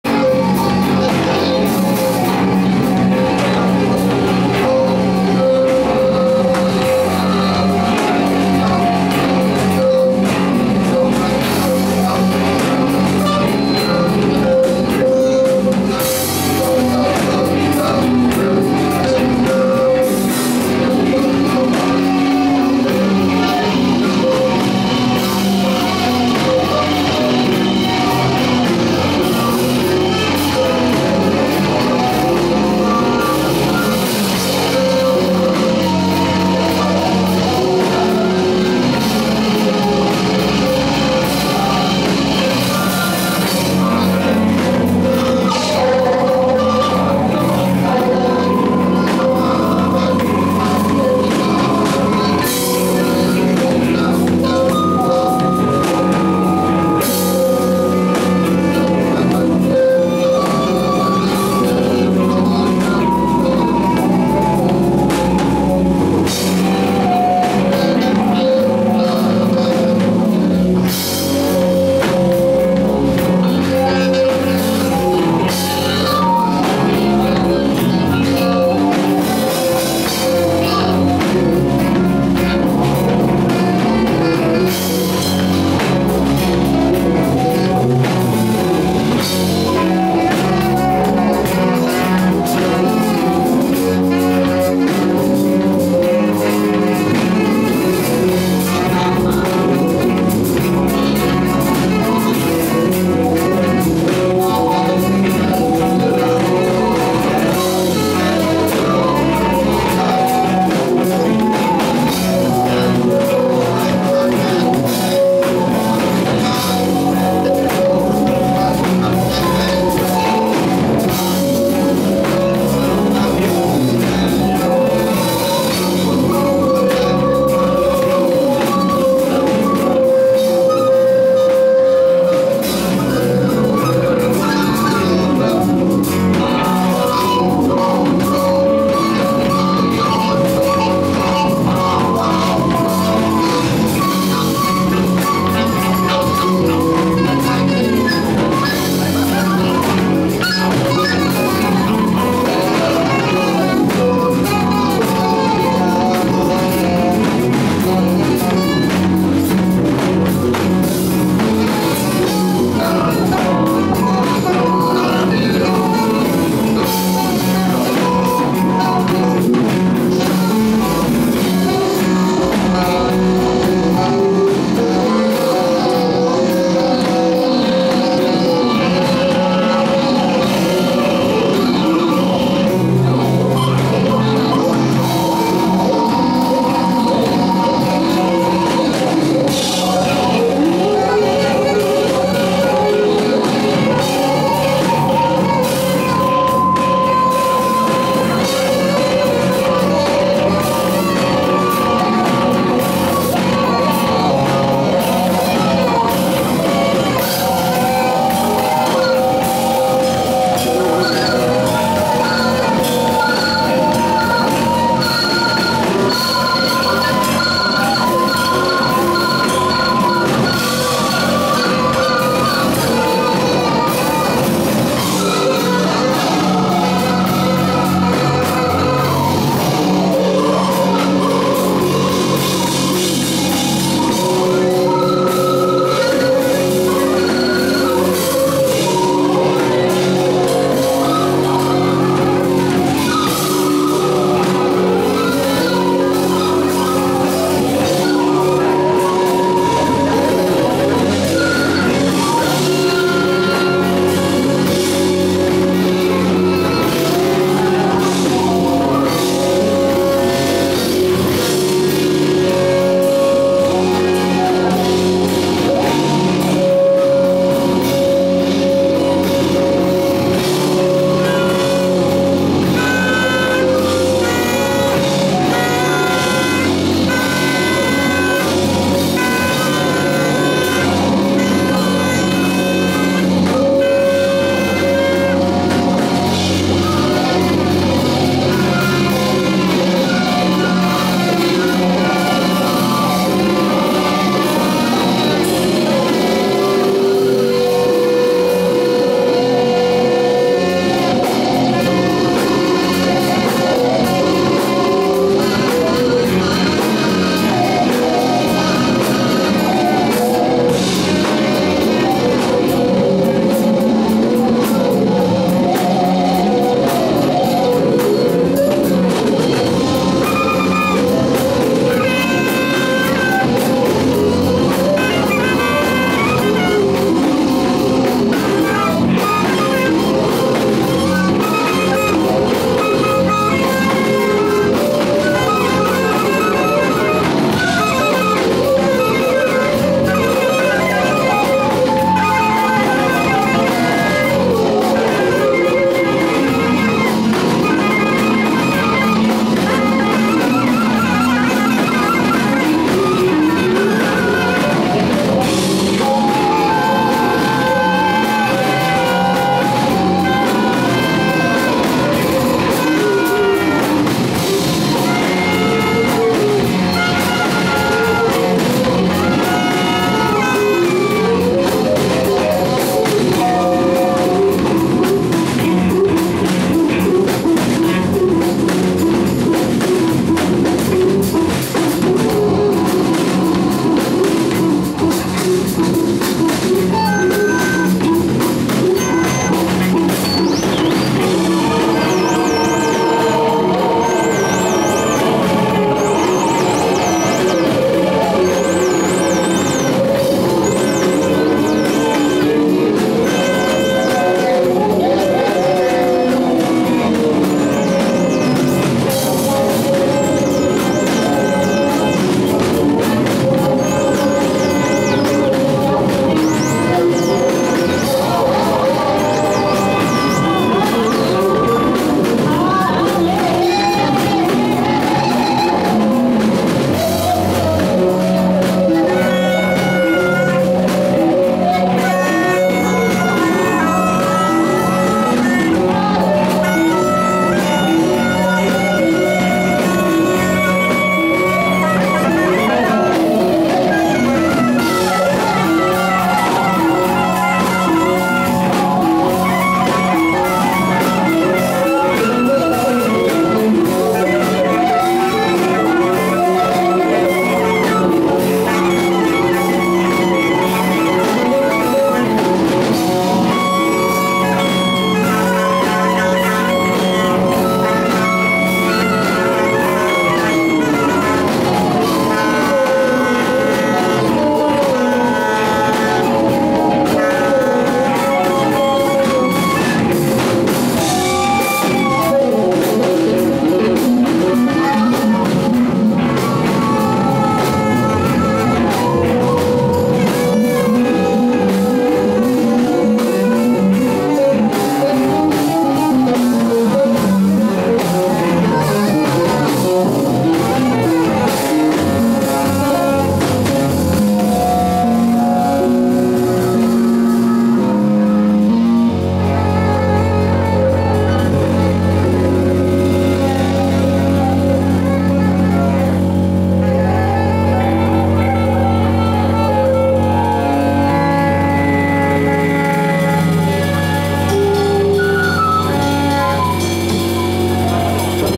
(8) - 차승민+최규철+송조지+나잠수+까르푸황+오건웅+함지혜+민상용+김오키+송조지+이태훈+윤석철/[OK Universe II :DEATH NOTE]/
_OK Universe II / DEATH NOTE:장실장,정지완,카멜,민상용,오건웅,송남봉,송조지,까르푸황,이태훈,김오키,함지혜,윤석철,나잠수,차승민,서좁곱,야마가타트윅스터,Nilsori,Saas
-주사위던지기로 3인의 순서가 정해지며 정해진 순서대로 돌아가며 무대위에 오름.그렇게 2~3시간을 온전히 버틴 자만이 살아남는다...